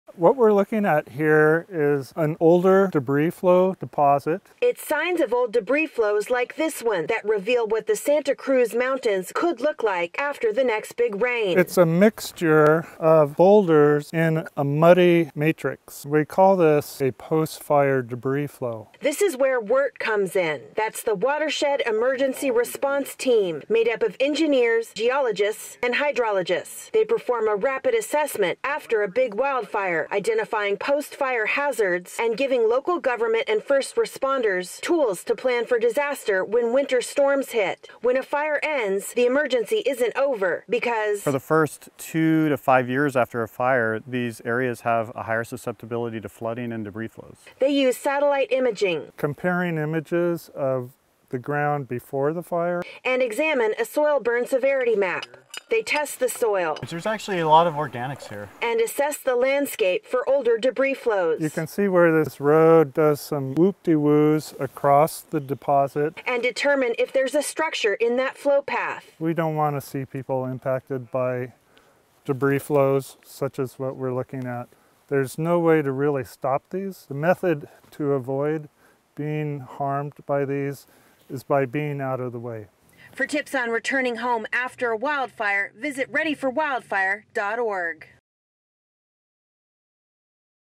WERT Assesses CZU Lightning Complex Burn Area
WERT, or Watershed Emergency Response Team, is a group of highly-trained engineers, hydrologists, and geologists that visit fire-damaged areas.  Their goal is to identify post-fire hazards.

For tips on returning to a home in or near a wildfire footprint, visit readyforwildfire.org.